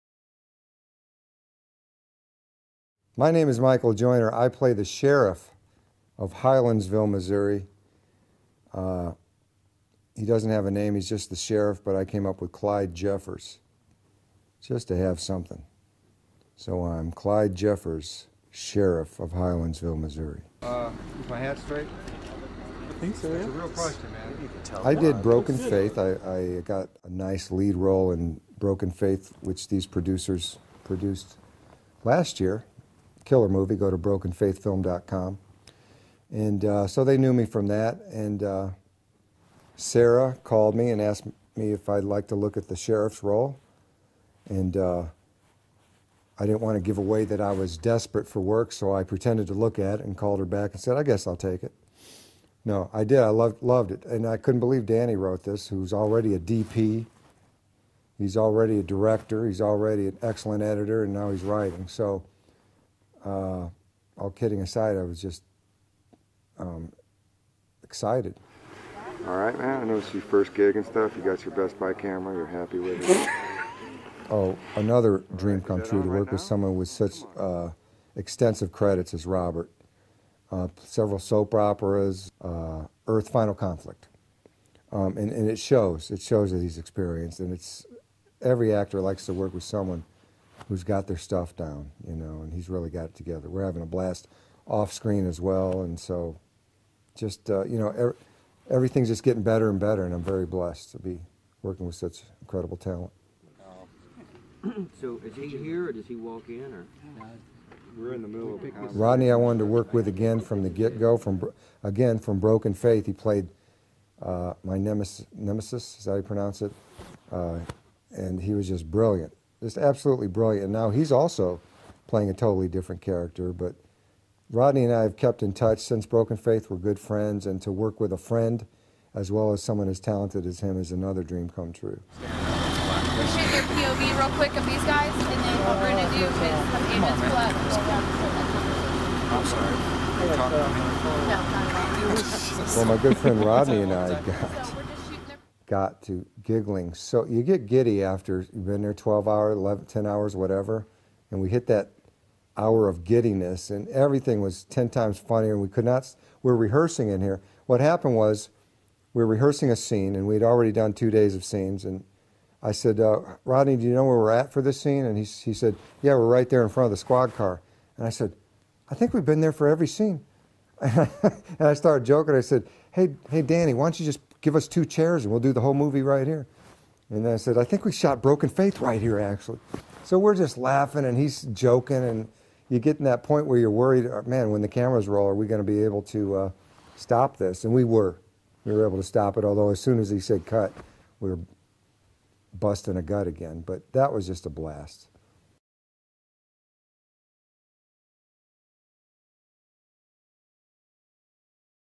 Surrender Actor's Interview: Michael Joiner
An Interview With Michael Joiner on the set of Surrender!
Property of Sunrise Pictures
Editor: Quintin Huckaby